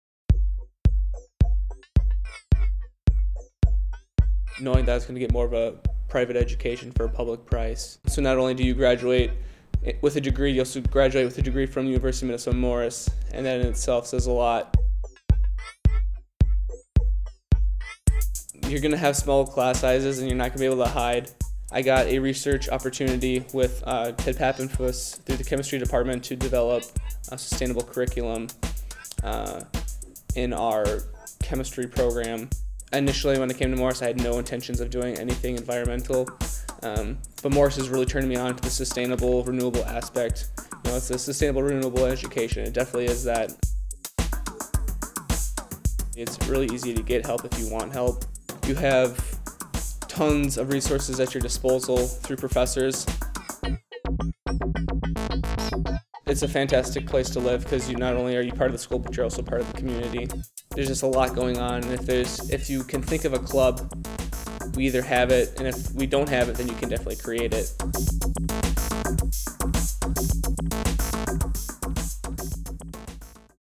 Why Morris?
Morris is a learning environment that renews, sustains, and inspires you for a lifetime.

Discover what a renewable, sustainable education means to you.

admissions.morris.umn.edu